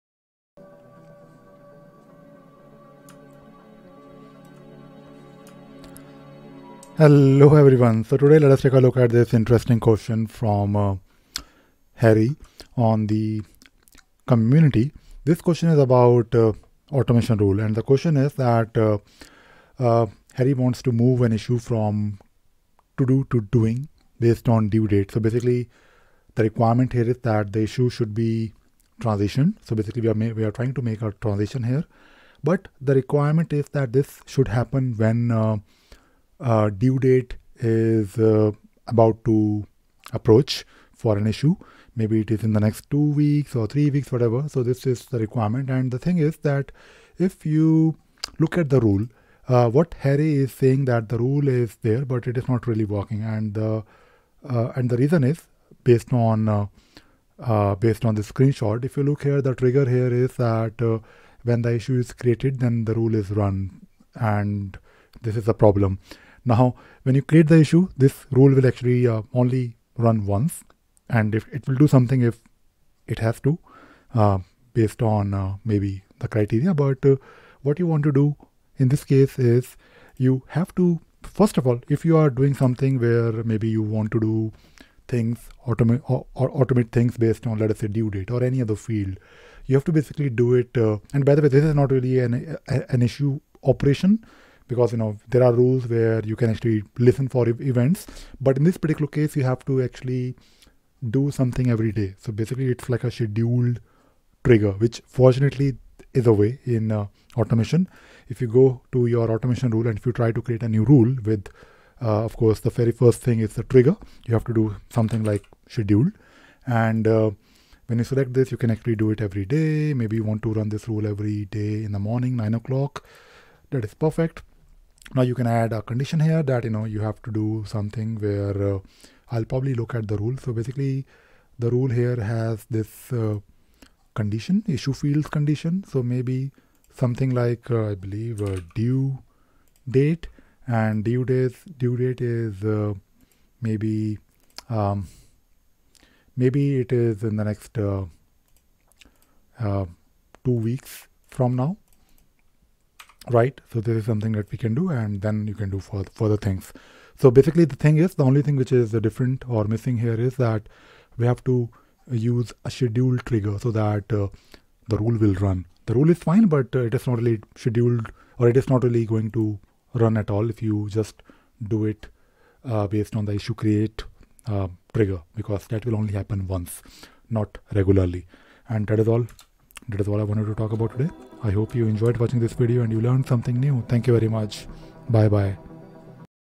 Jira Cloud Automation - Why my rule is not working?
Want to transition the issue based on due date but the rule is not working? Let us find out.